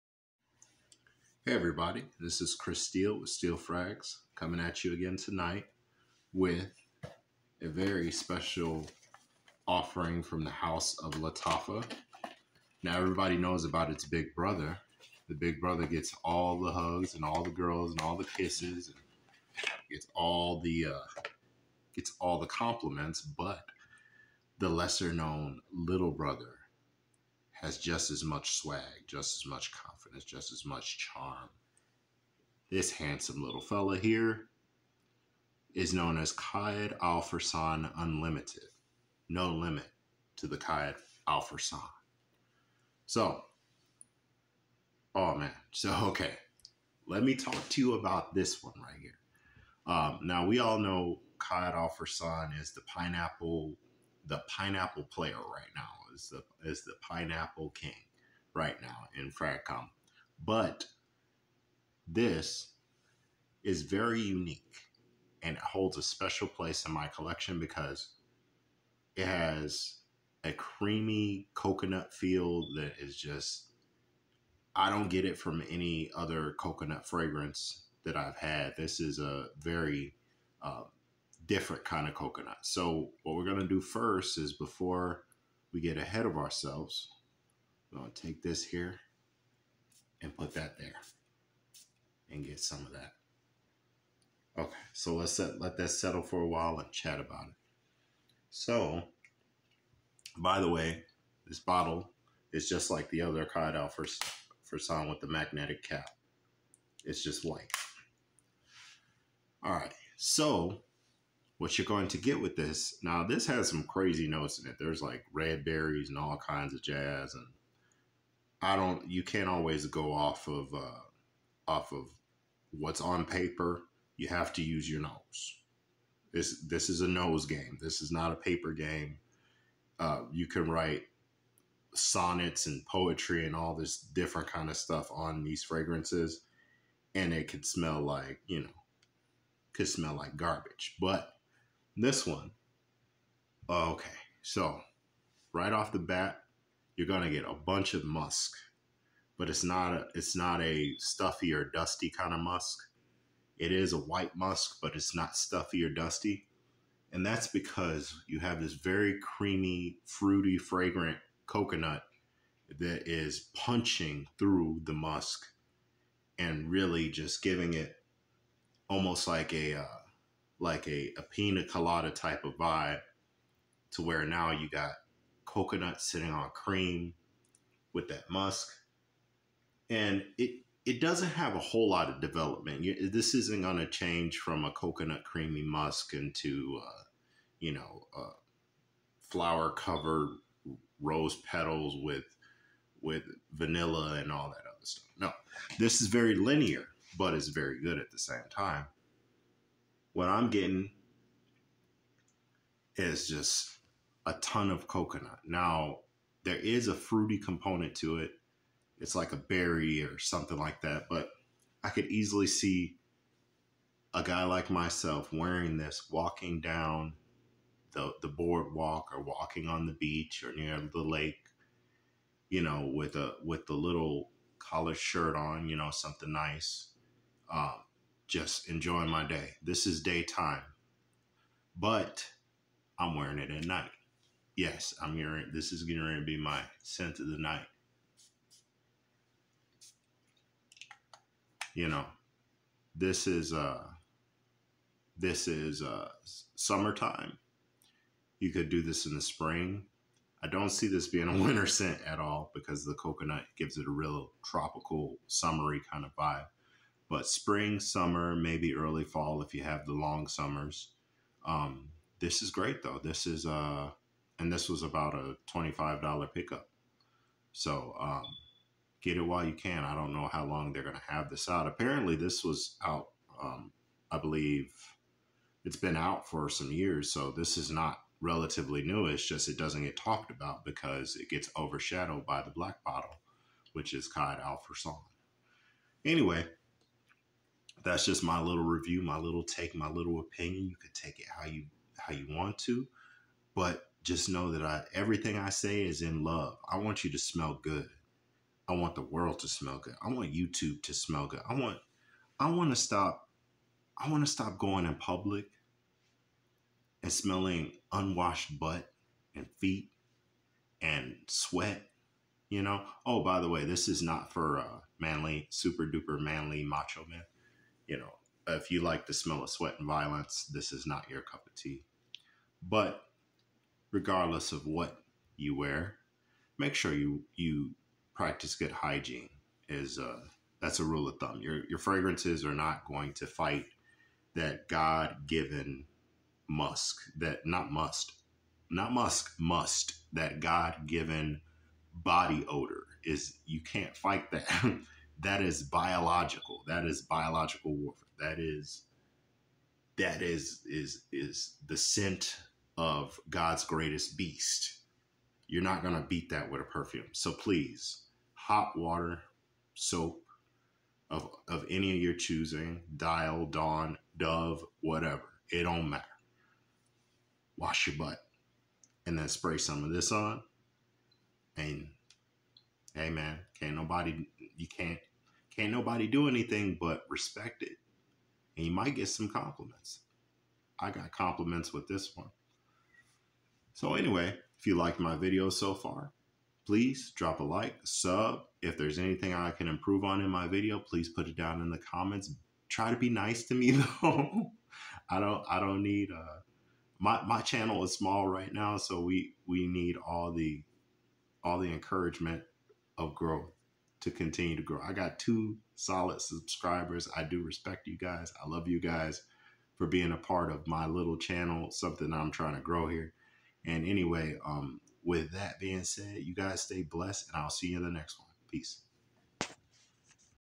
LATTAFA Qaed al Fursan Unlimited sweet creamy coconut for vibin in the summer🥥fragrancereview
Sweet coconut meets musk in a really nice way. Easy to wear and nice for the price. With decent performance there's very little to not like.